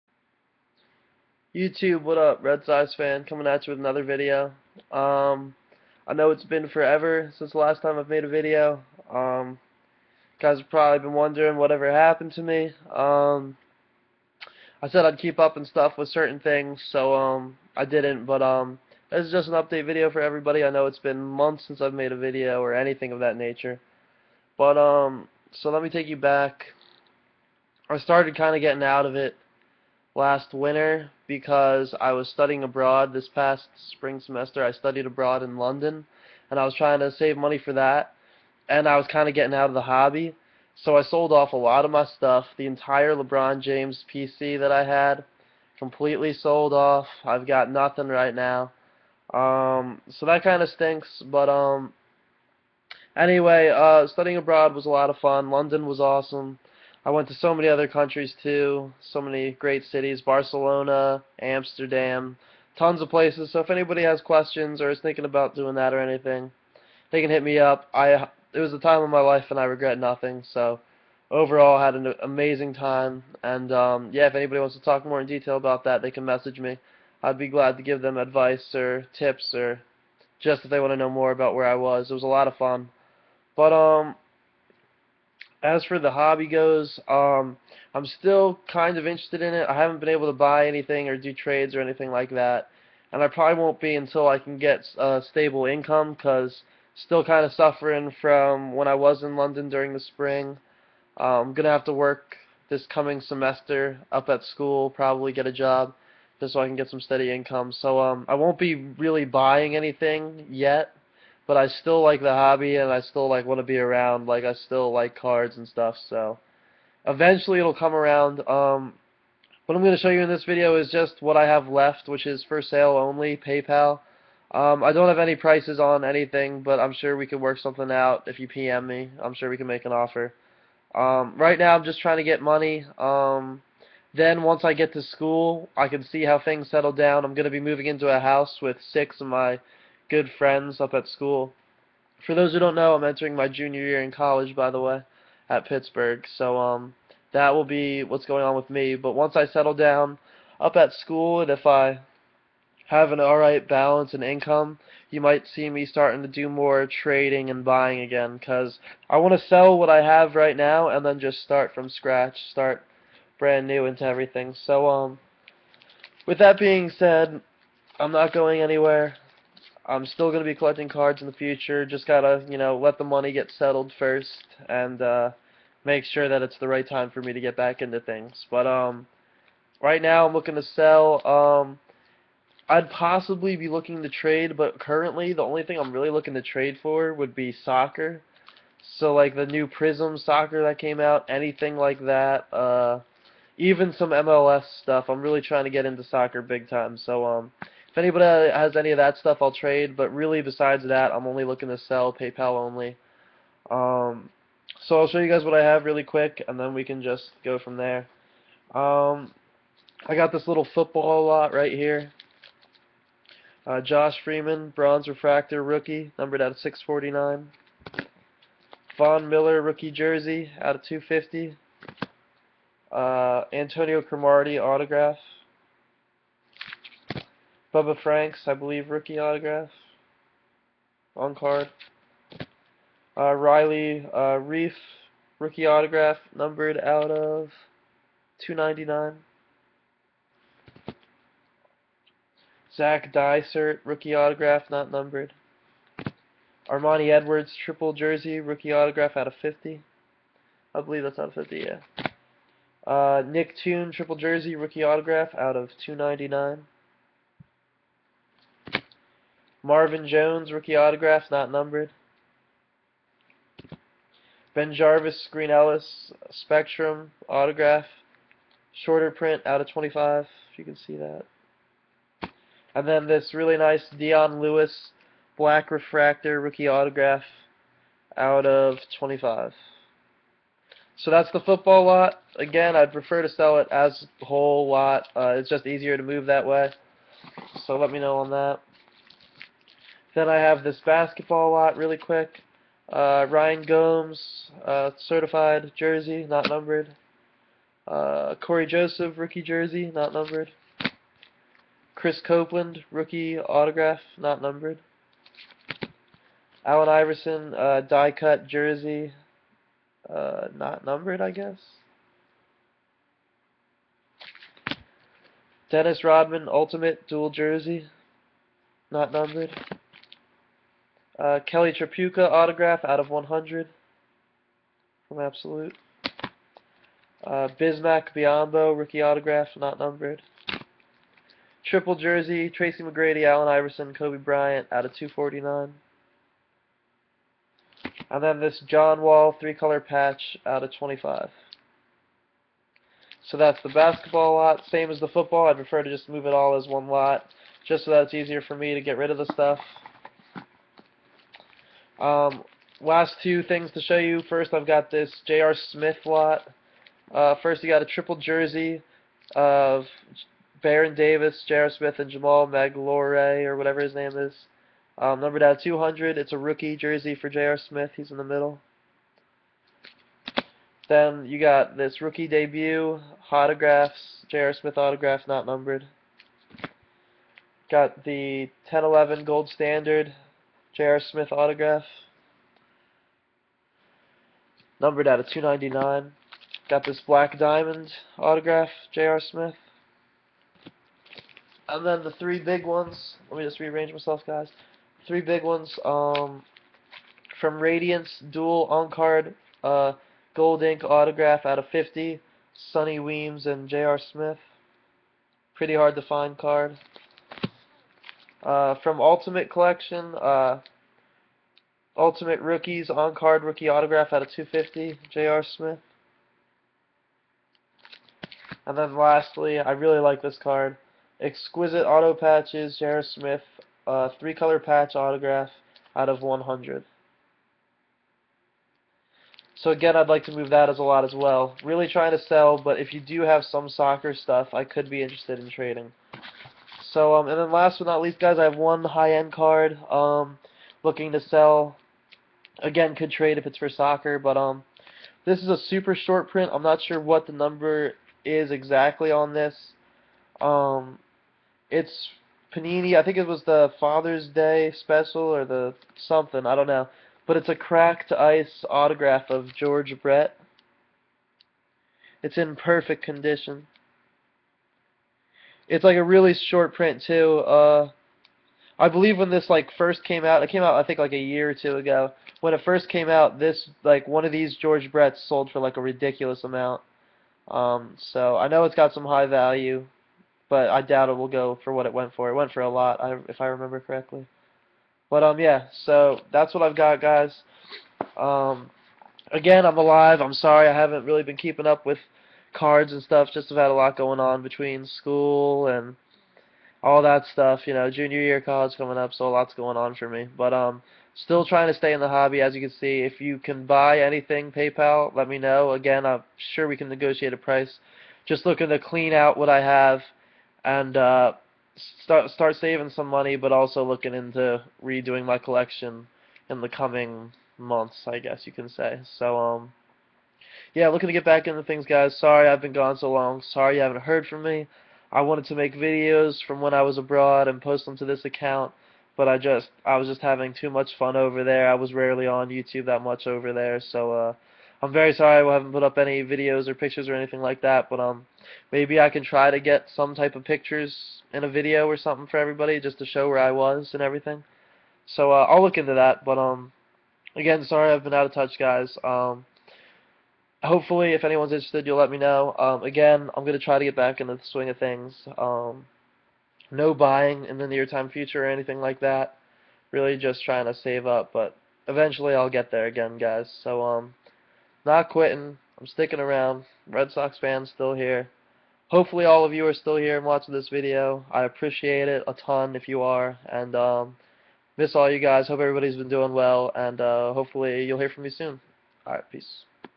I'M ALIVE! Studying Abroad, Future In The Hobby, Cards For Sale
Again, sorry I haven't kept in touch with anyone, but miss you all and thank all of you for your support! I'm not going anywhere!